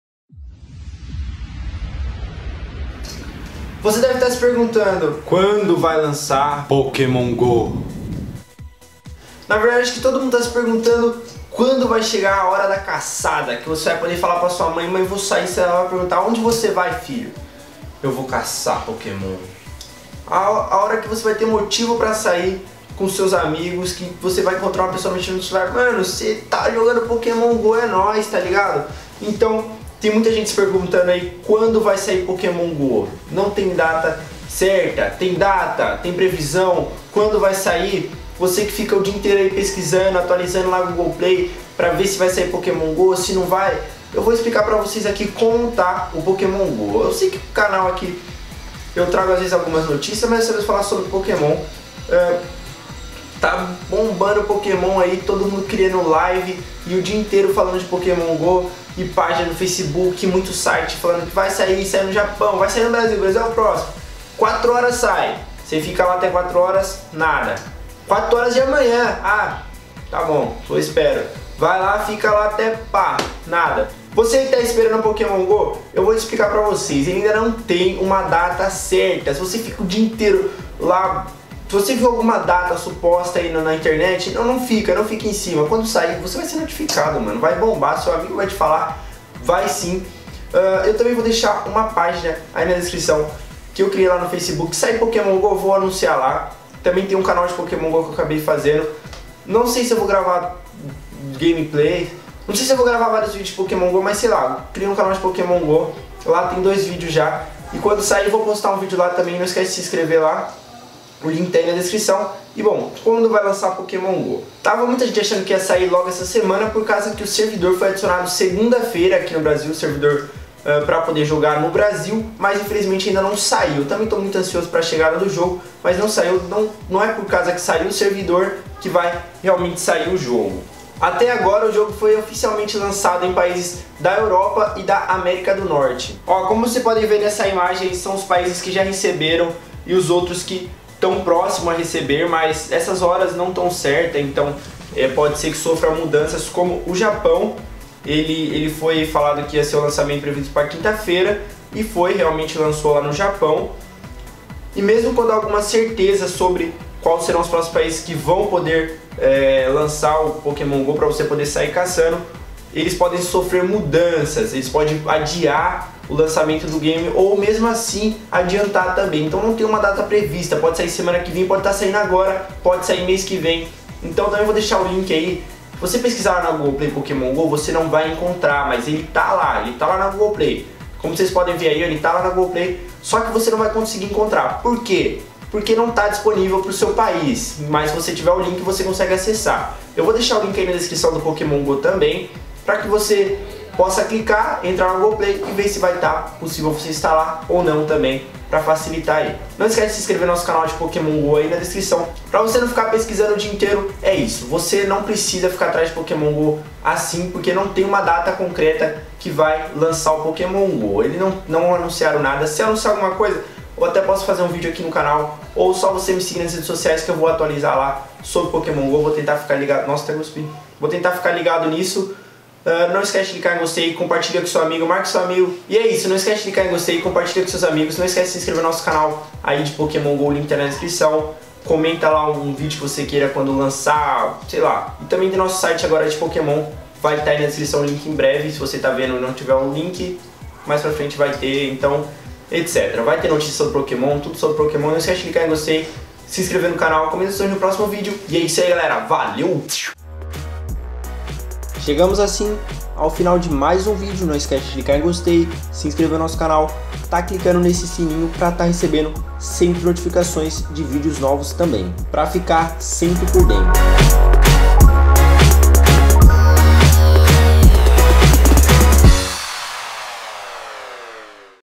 Afinal quando vai lançar Pokémon GO no Brasil?
Confira todos os link abaixo para ficar ligado quando vai sair o game, e o que falo no vídeo é que não tem uma data certa o que você pode fazer é somente esperar infelizmente :/